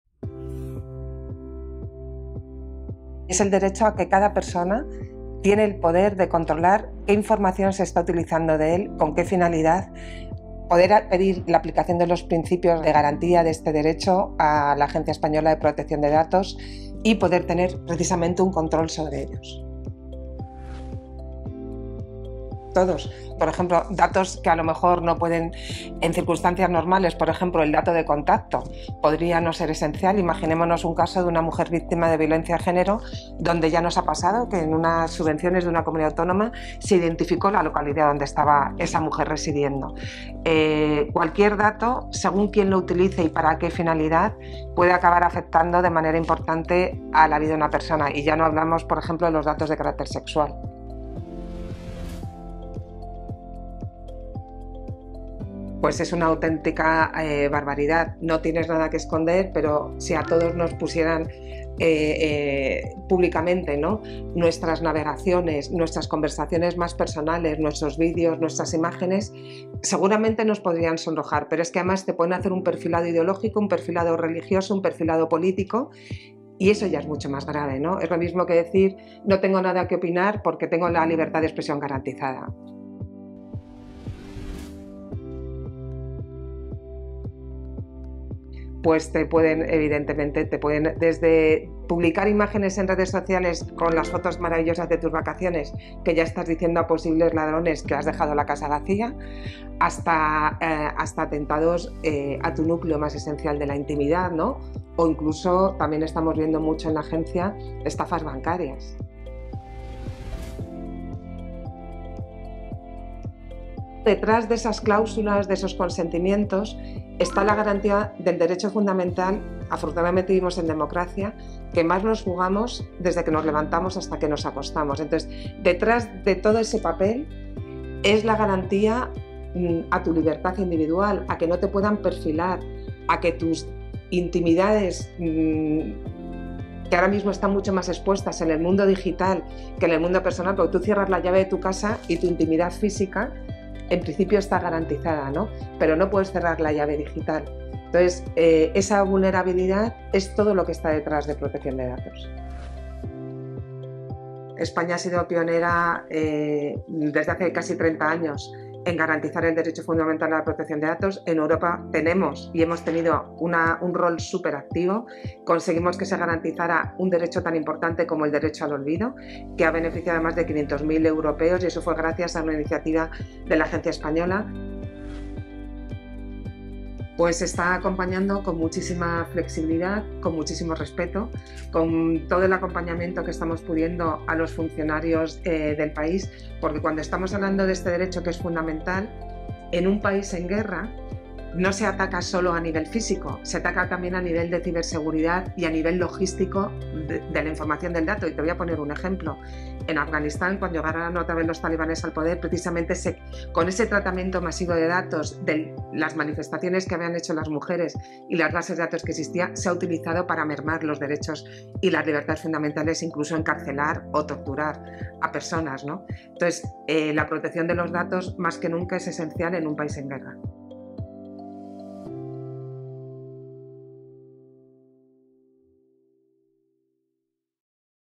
¿Qué es protección de datos? | FIIAPP
España, impulsora, entre otros, del derecho al olvido, es pionera en protección de datos. Contamos con una de las autoridades públicas más activas en el mundo para promover la defensa de este  derecho fundamental, la Agencia Española de Protección de Datos.

El conocimiento y la experiencia de nuestras instituciones públicas en esta materia está en el centro de dos importantes proyectos europeos de cooperación internacional con los que la FIIAPP está facilitando, en 2023, la participación de profesionales de la Agencia en el exterior.